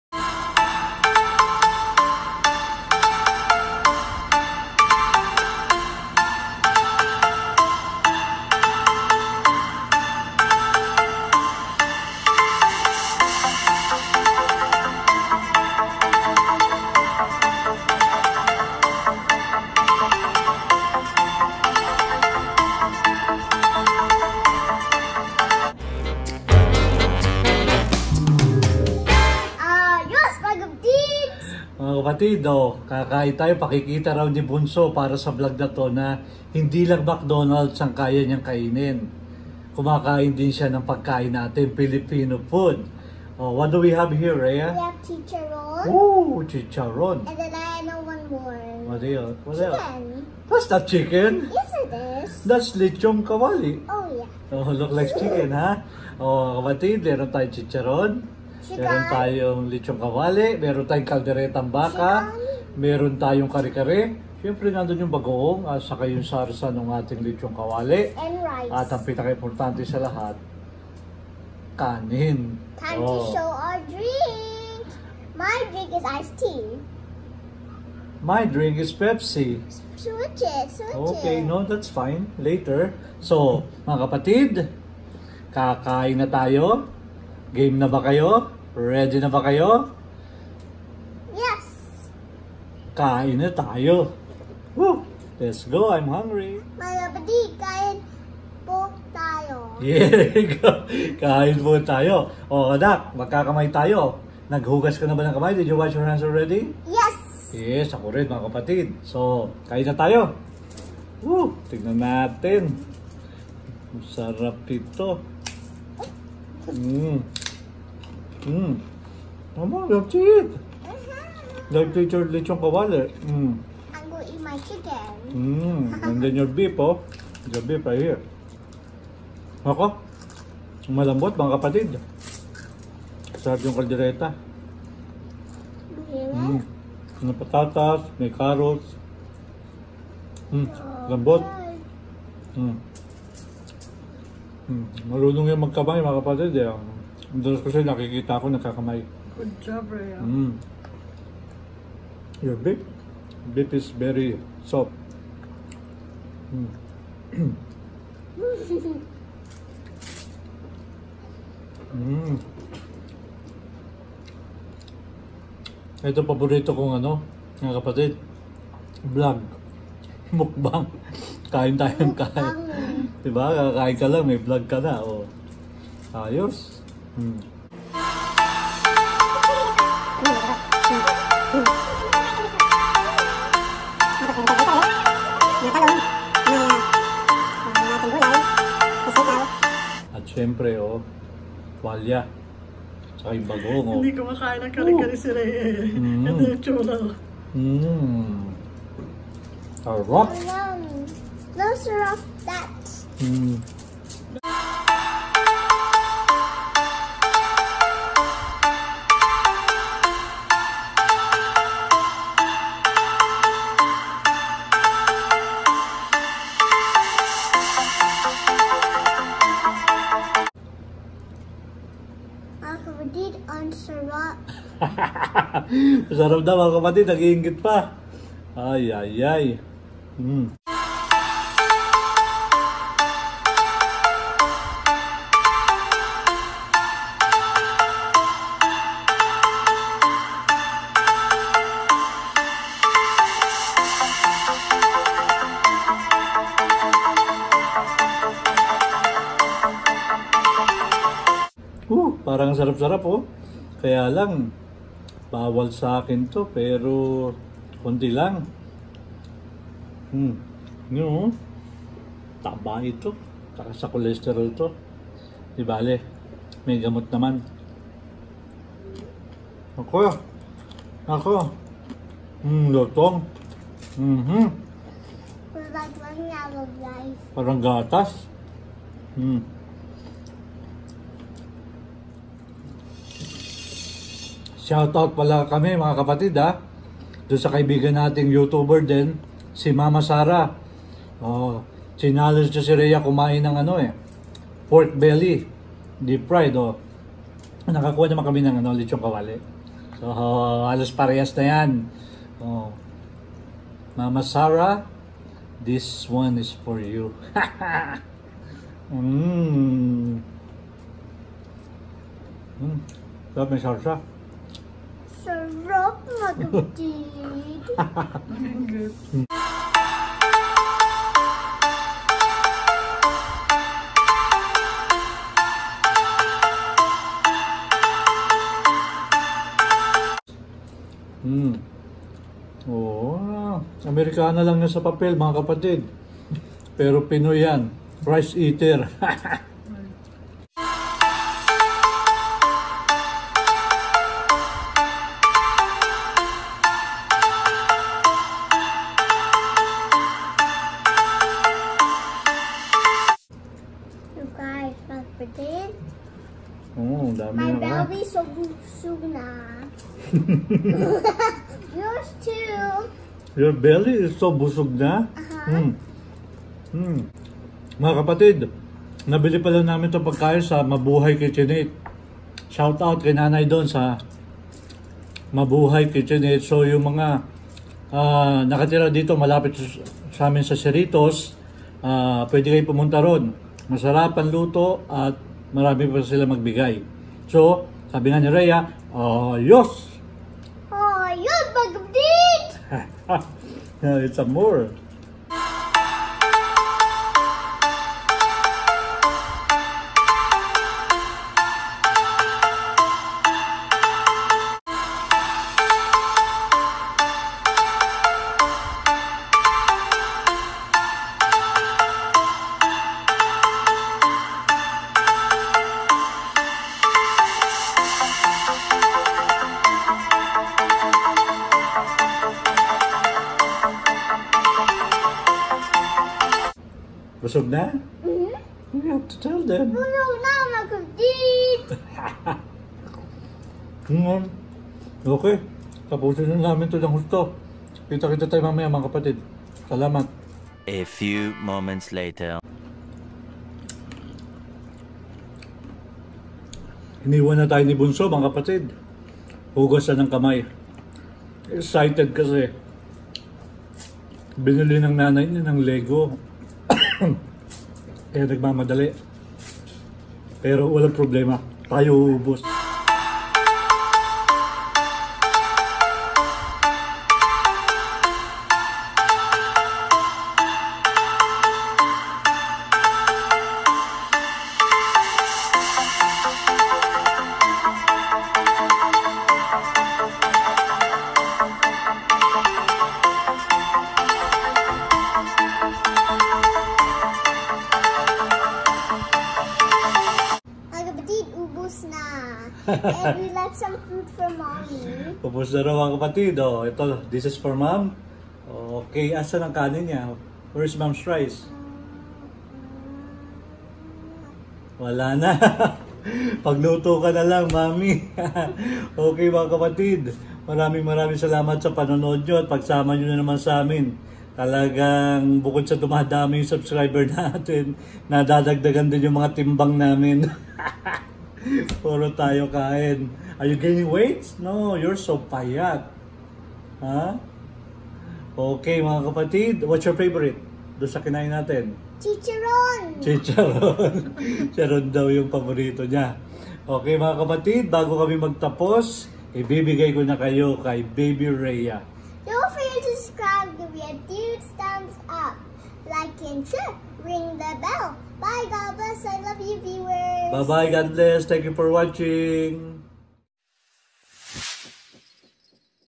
Pinoy Food Mukbang with Bunso
Filipino food mukbang featuring lechon kawali, karekare, kaldereta, and chicharon